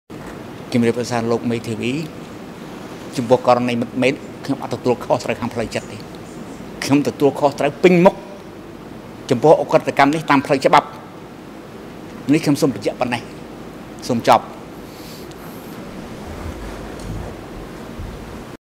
Statements of apology 09 July 2009: Kaing Guek eav
In its judgement on appeals in Case 001 against Kaing Guek Eav alias Duch, the Supreme Court Chamber of the Extraordinary Chambers in the Courts of Cambodia (ECCC) affirmed the Trial Chamber's previous decision to compile and post on the ECCC's official website all statements of apology and acknowledgements of responsibility made by Kaing Guek Eav alias Duch during the course of the trial, including the appeal stage. This video clip is part of the audio and video version of this compilation.